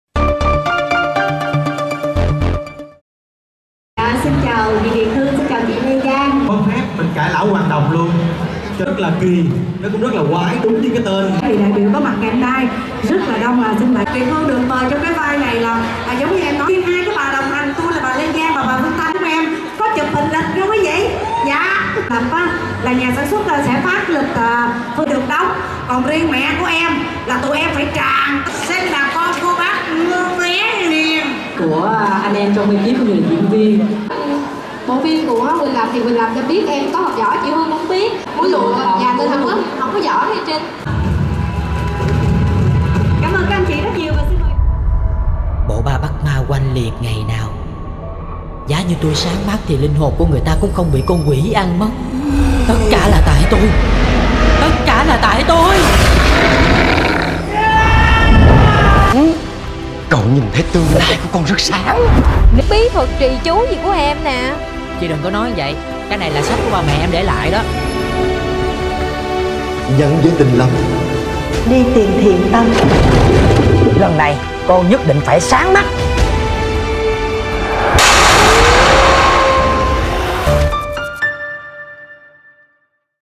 Đông đảo sao Việt chúc mừng Huỳnh Lập ra mắt phim điện ảnh đầu tay
Đông đảo sao Việt và người hâm mộ đã đến chúc mừng êkip phim Pháp sư mù có buổi công chiếu tại TP.HCM. Đây là sản phẩm điện ảnh đầu tay của Huỳnh Lập, do anh và Lý Minh Thắng đảm nhận vai trò đồng đạo diễn.